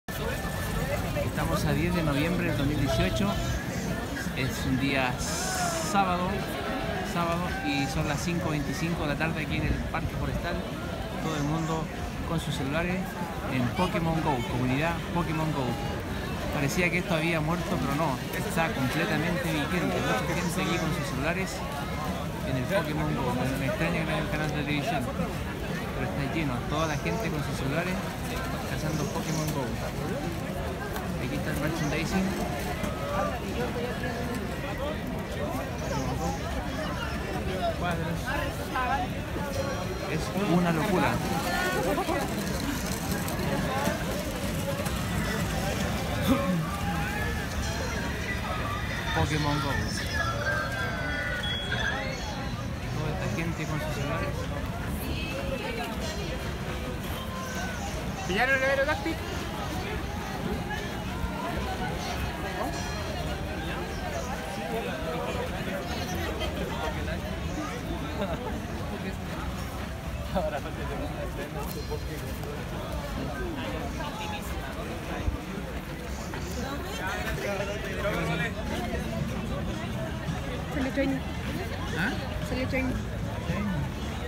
Comunidad Pokemón Go con Vicente Cosio
Sábado 10 de Noviembre 2018 en el Parque Forestal vs MAC cazando Pokemones